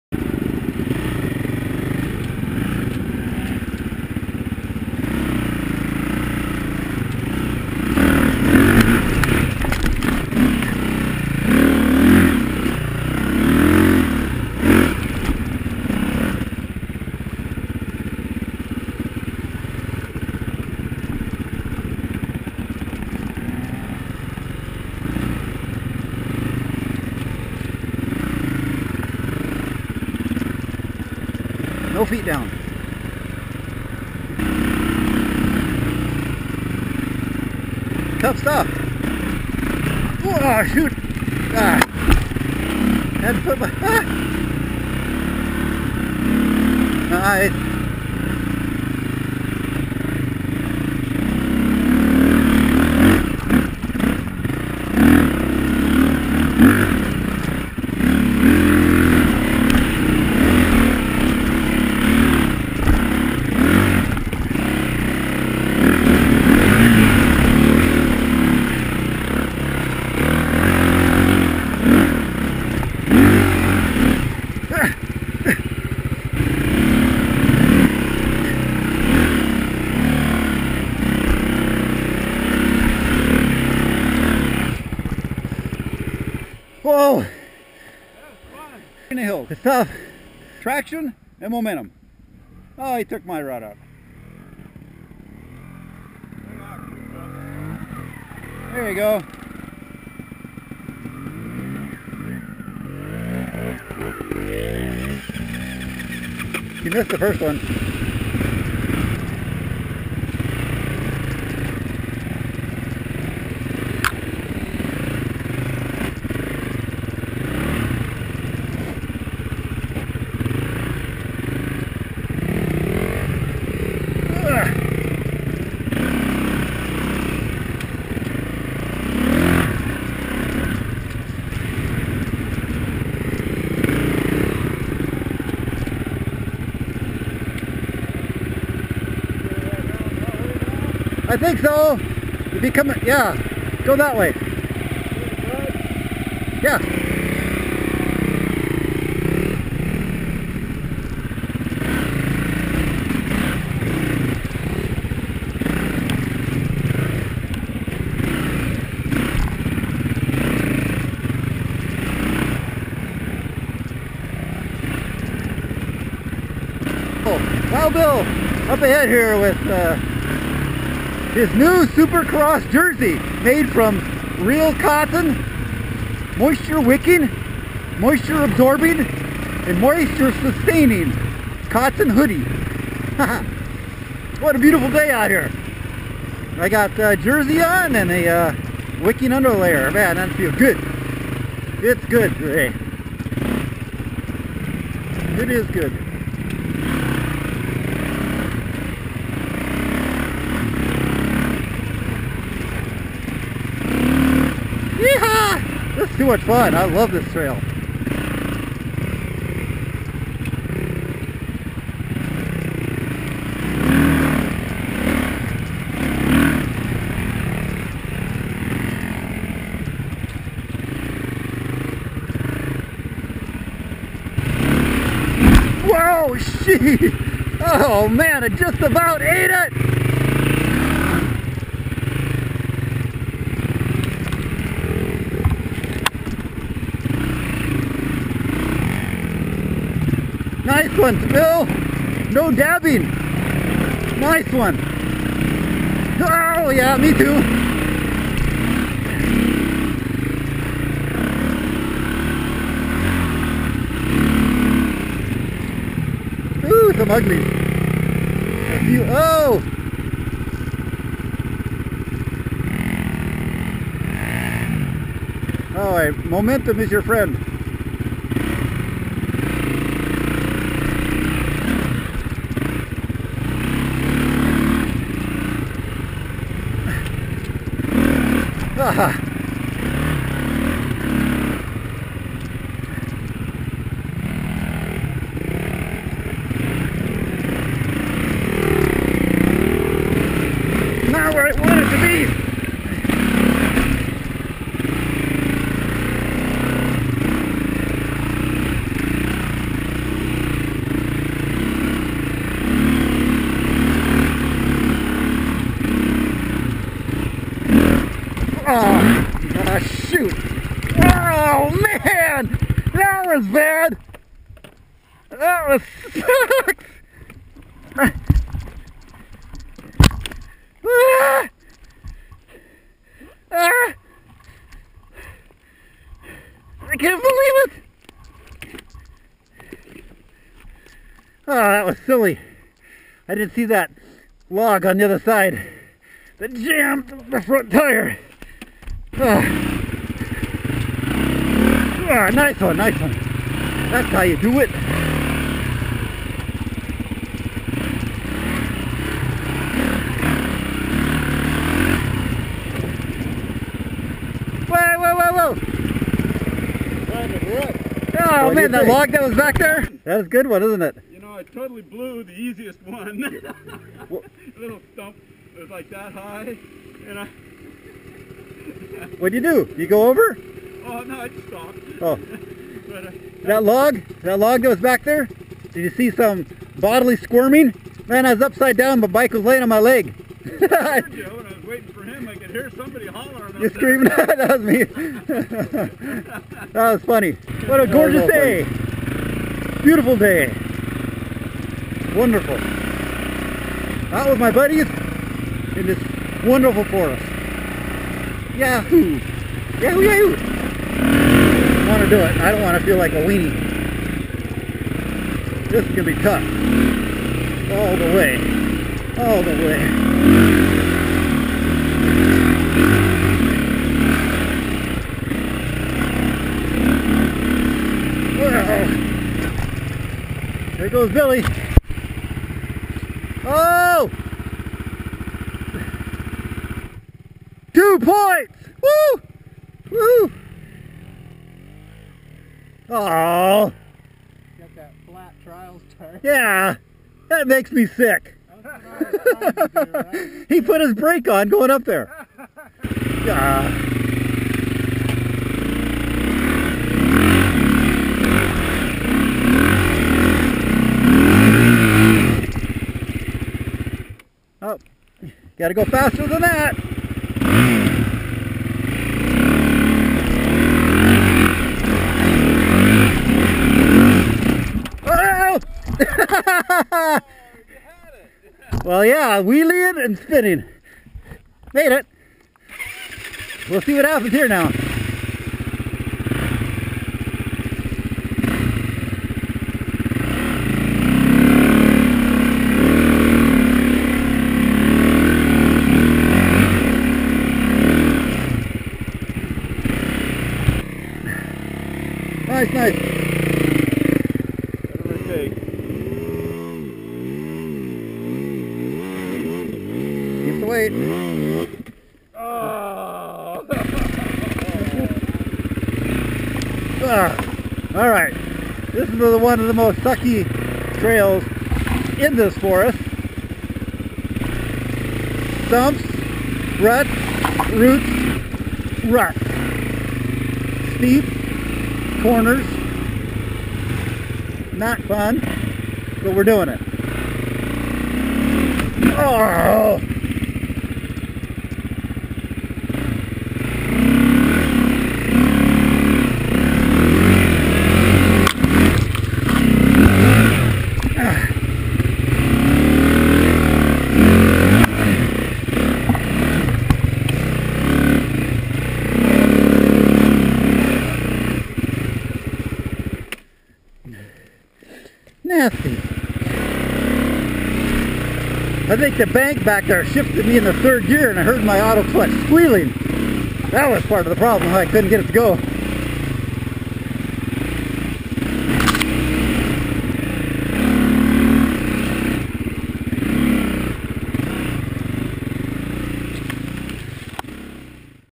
Gnarly2 Highlights
More fun,  More crash, More laughs and one guy riding an Ossa Magic Carpet.